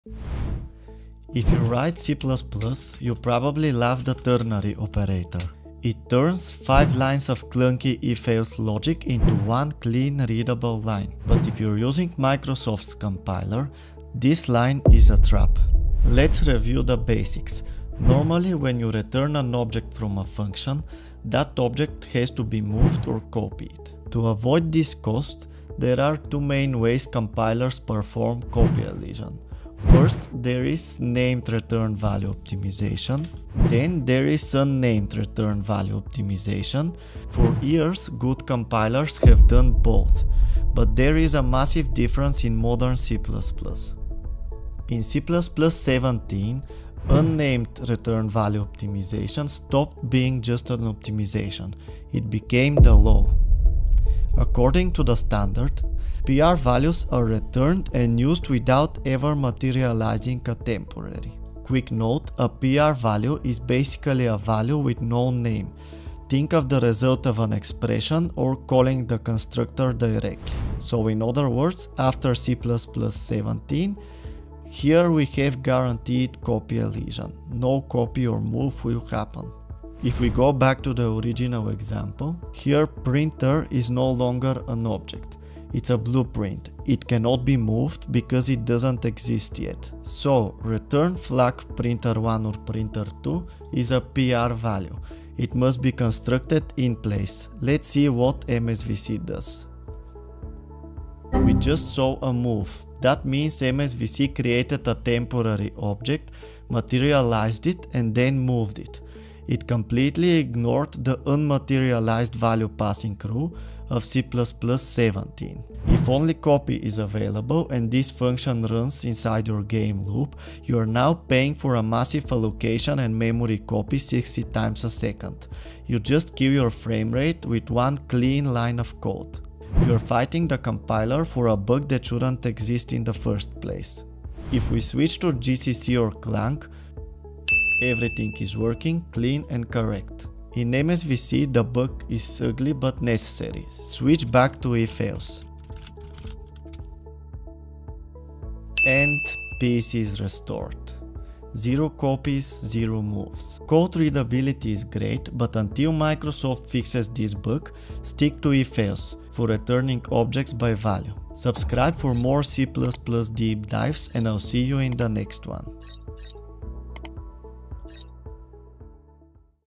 This "Clean" C++ One-Liner Kills Performance
Did you know the ternary operator can silently break C++17 Guaranteed Copy Elision? In this video, we expose a critical MSVC compiler bug where returning a prvalue via the ternary operator invokes a hidden move constructor instead of performing Unnamed Return Value Optimization (URVO).

We are taught to write "clean" code, but sometimes concise one-liners trap us. Since C++17, the standard mandates "Unmaterialized Value Passing" for prvalues, meaning temporary objects shouldn't even exist until they reach their final destination. This guarantees zero-cost returns.

However, the Microsoft Visual C++ compiler (MSVC) fails to respect this rule when using the ternary operator (? :). Instead of constructing the object in place, it creates a temporary, materializes it, and forces a Move operation.

If you are working with non-movable types like std::mutex or std::unique_ptr, this code won't compile. If you are a Game Developer, this "clean" line could be triggering expensive copies 60 times a second.